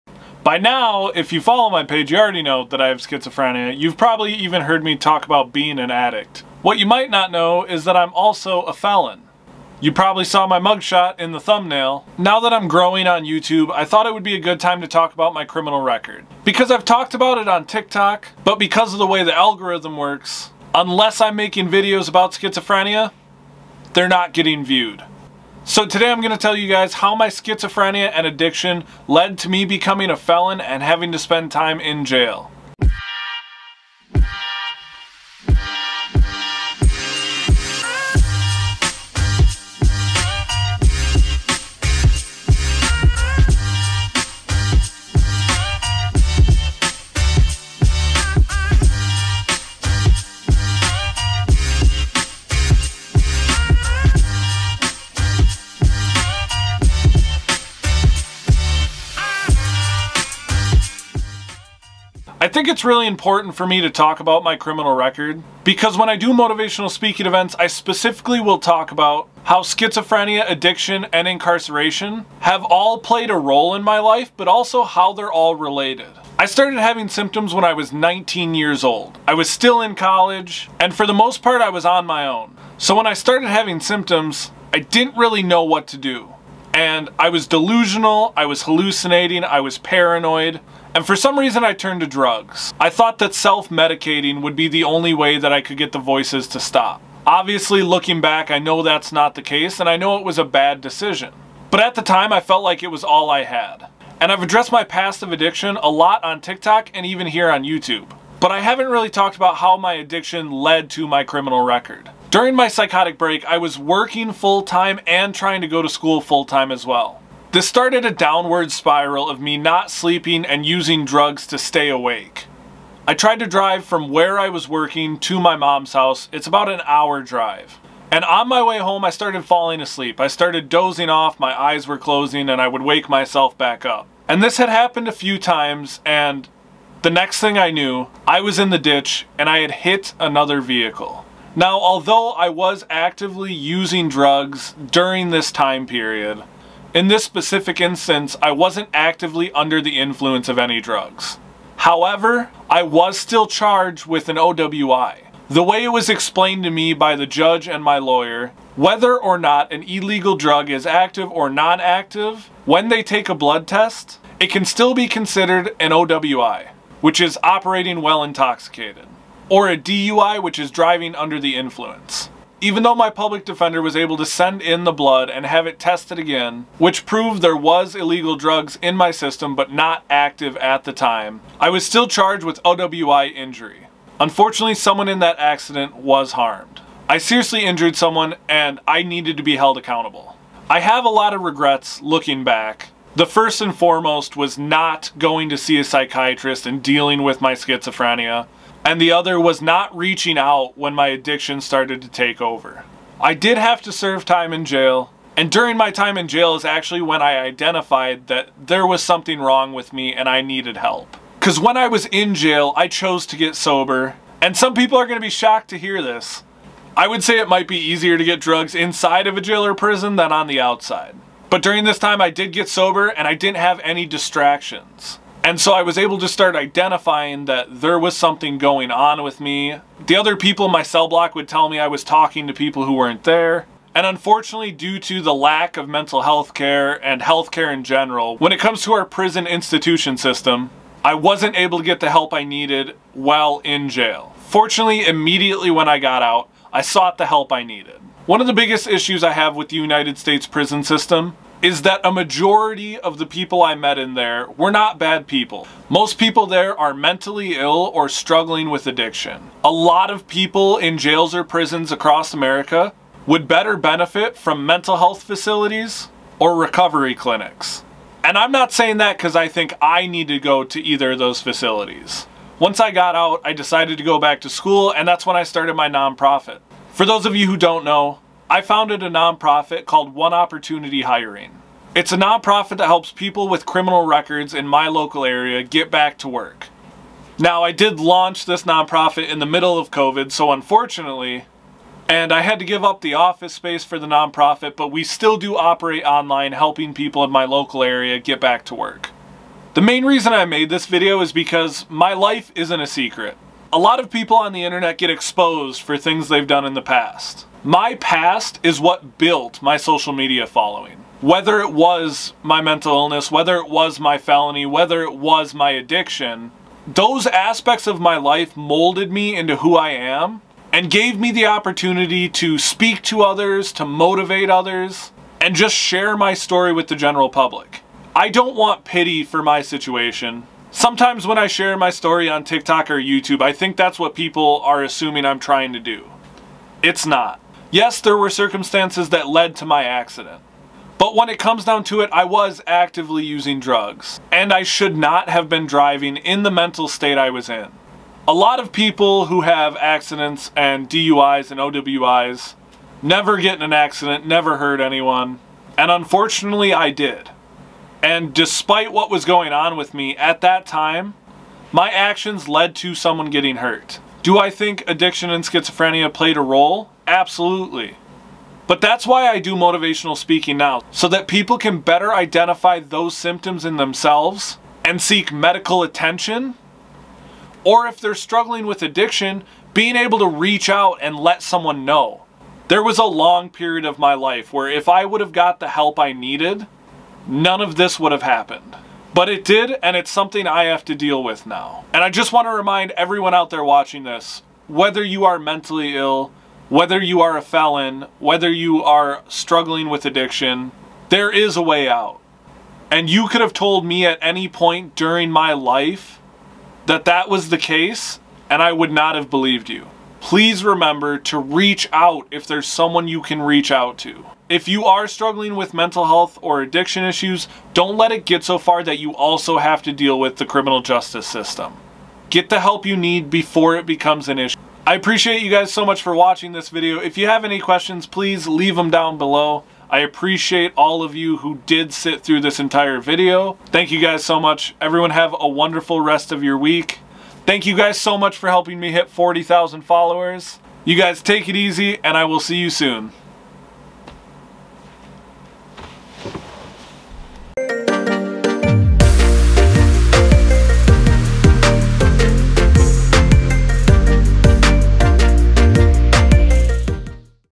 How I became a FELON
In this video I wanted to discuss my CRIMINAL RECORD. I want to be clear, I take full responsibility for what happened DESPITE my mental illness. I am not trying to push blame or claim innocence. However, schizophrenia and addiction did play a role in my criminal record. Watch to learn more!